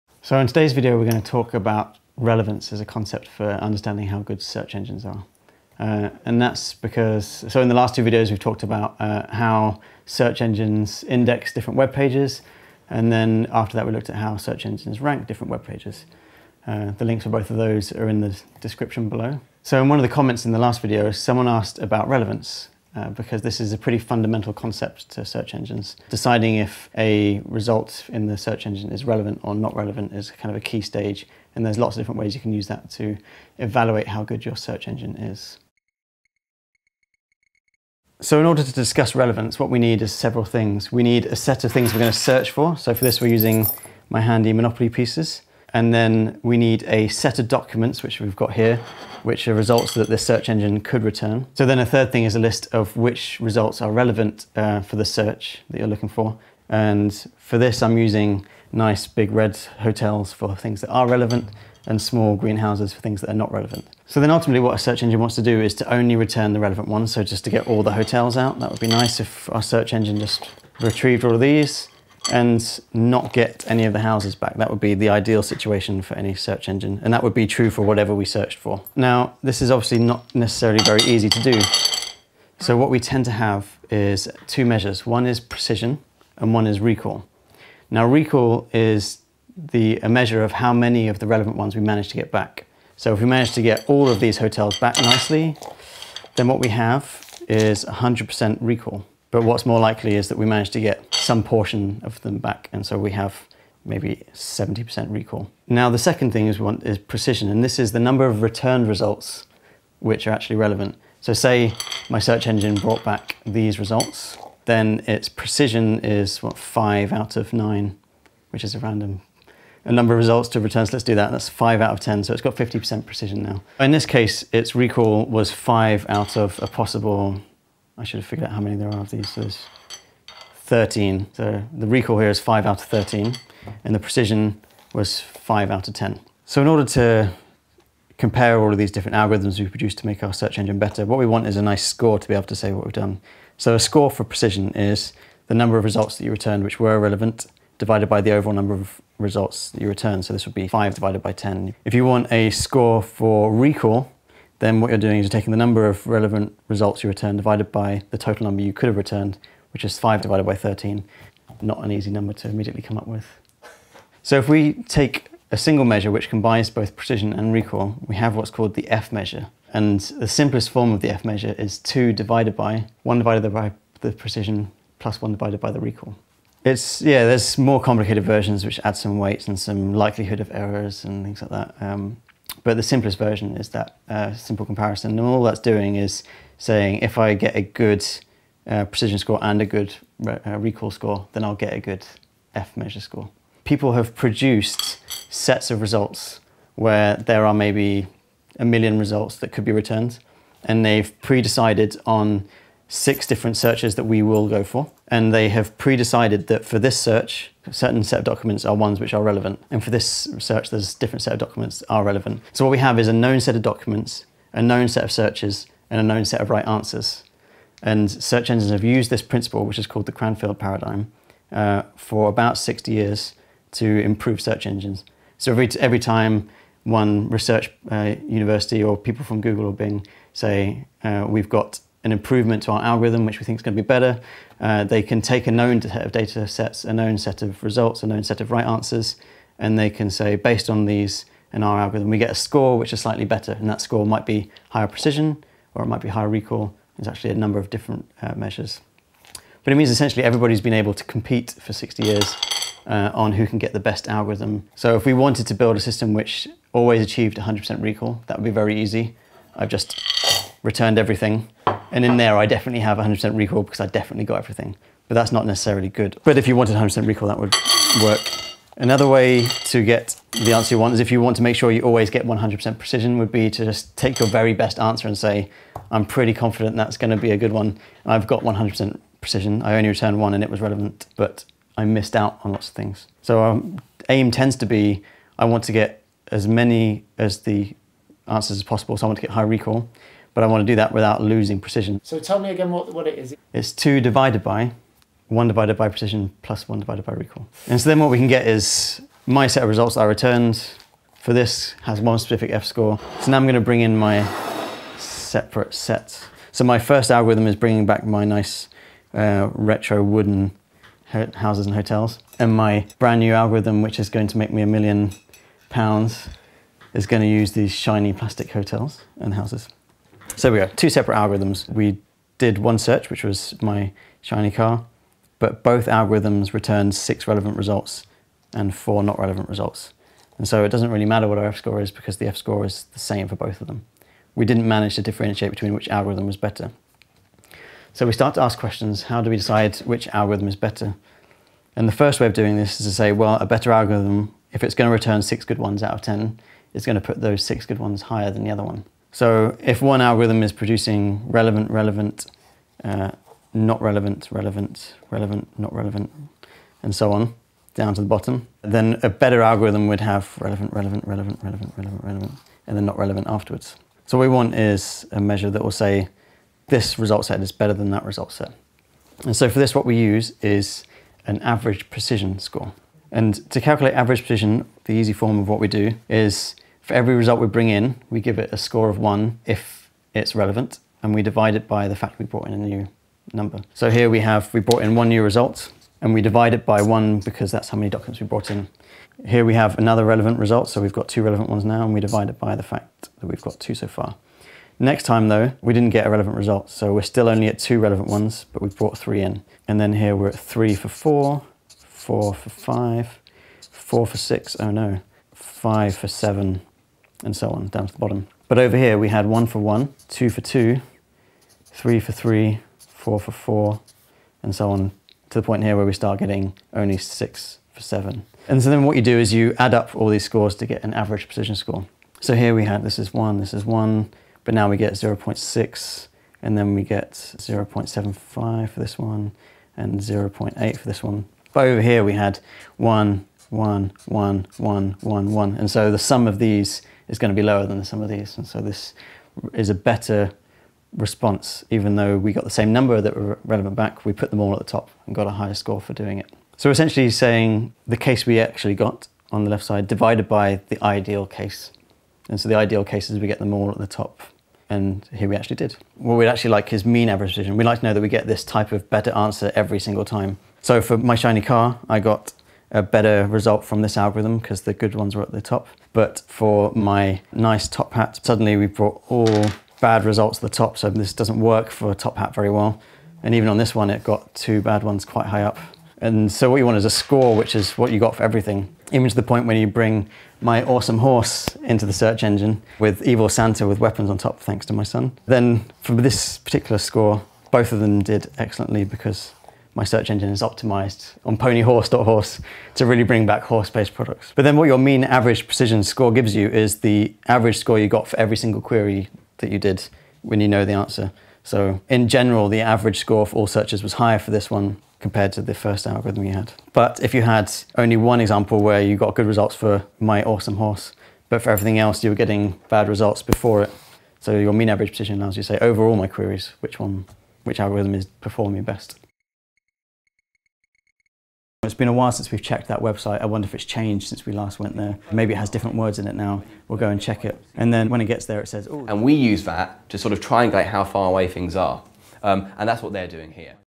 Search Engine Relevance - Computerphile
A search engine can return thousands of web pages, but how does it know whether they are relevant or not? First step is how to measure relevance, as Dr Max Wilson explains.